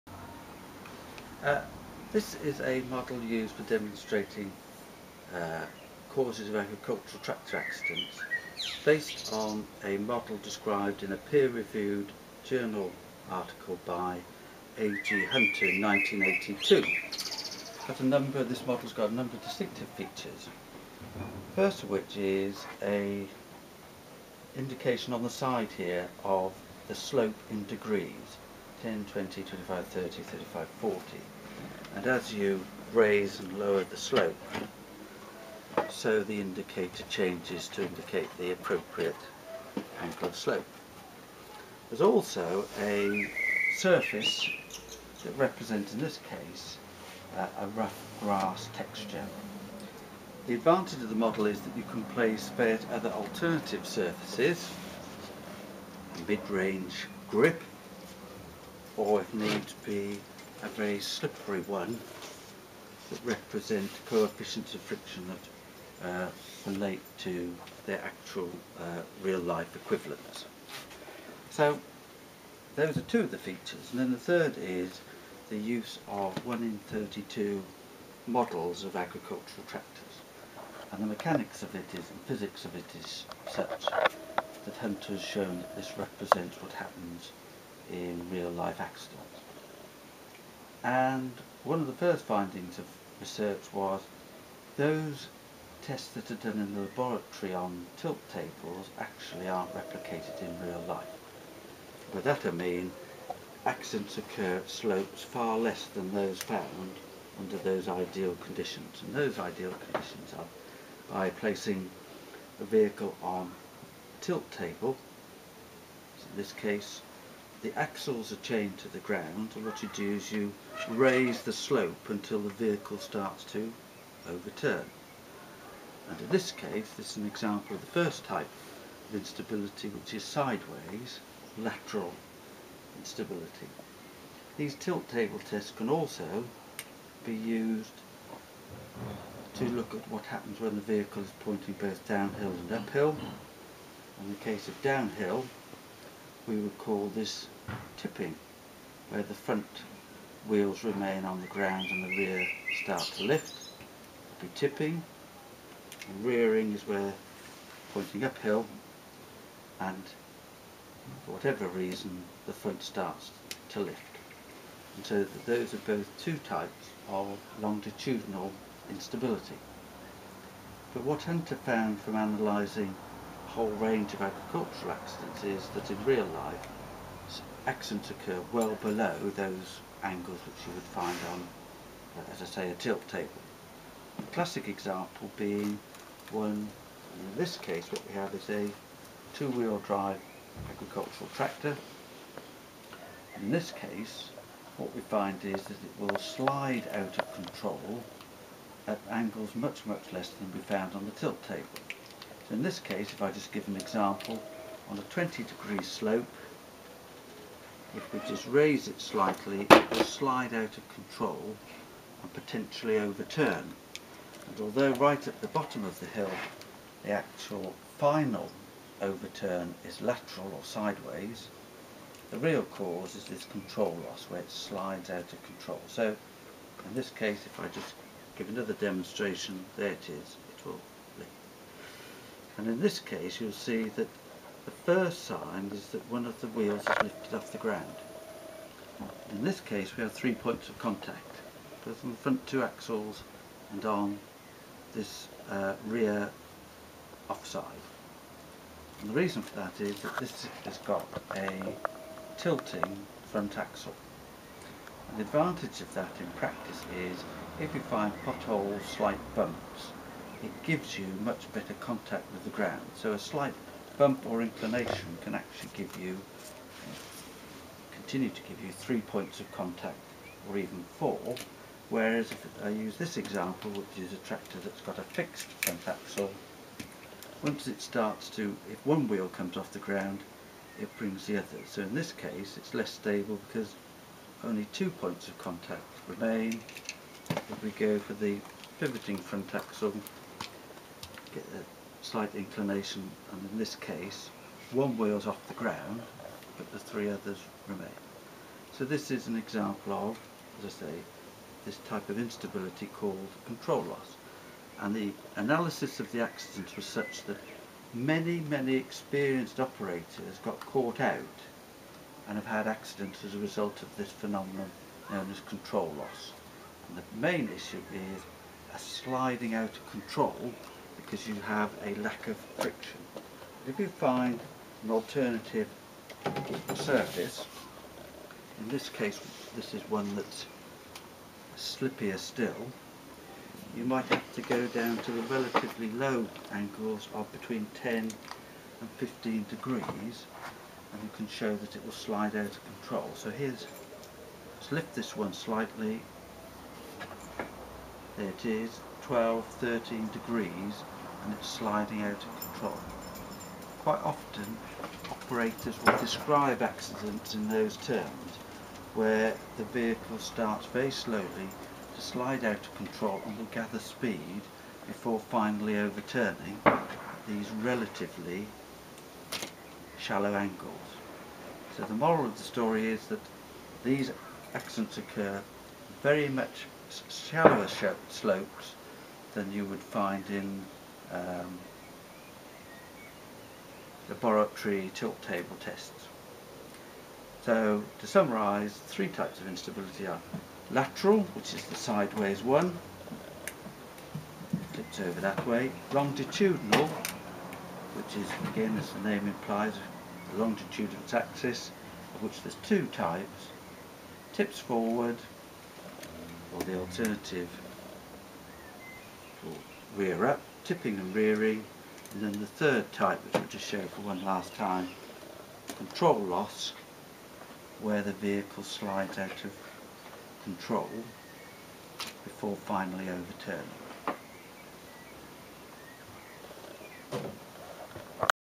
Tractors Falling Over - A Model for Demonstrating Agricultural Tractor Accidents
This clip describes a model based upon the work of AG Hunter in the 1980s.

The characteristics are a sloped surface which can be interchanged to represent different friction/grip, together with a sliding scale showing the slope in degrees.

Finally, the use of 1:32 scale models to varying represent tractor types.

Both longitudinal and lateral instability are demonstrated plus control loss .